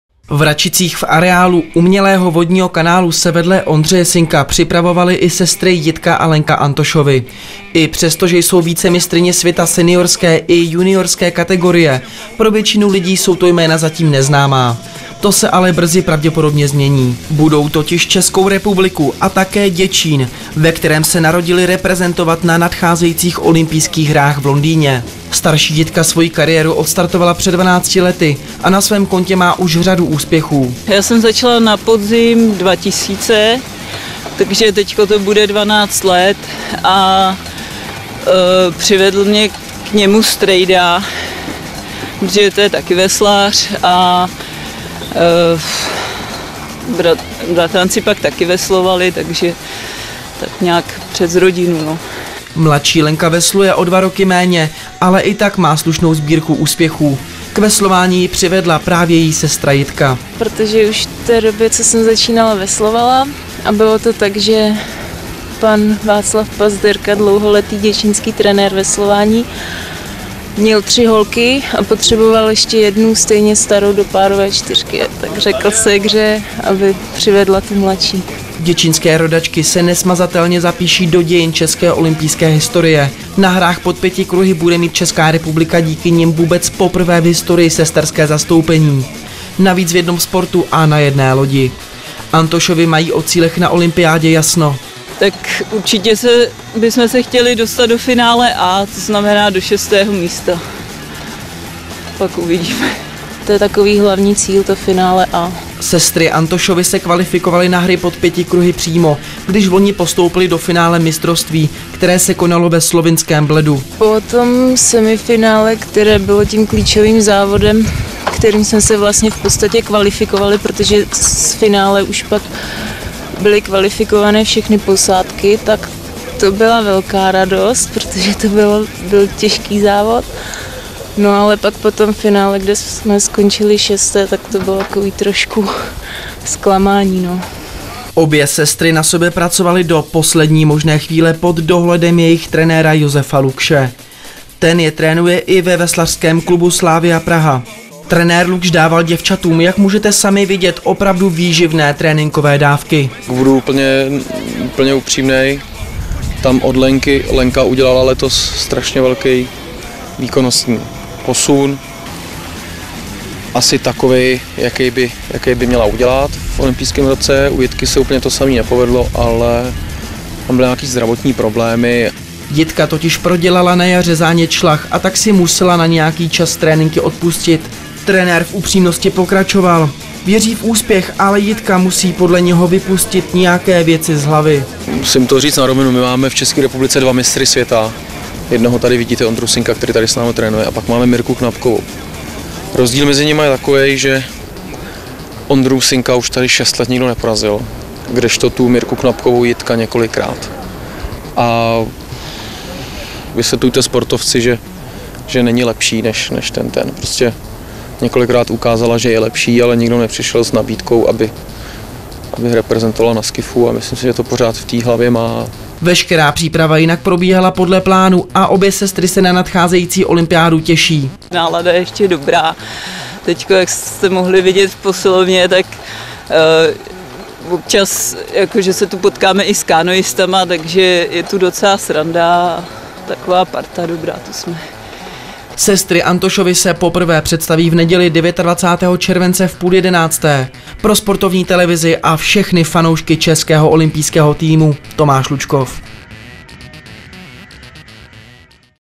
Antošovy: Chceme do finále!
Reportáž sportovni.tv. Sestry Antošovy, Jitka a Lenka, příprava před OH 2012.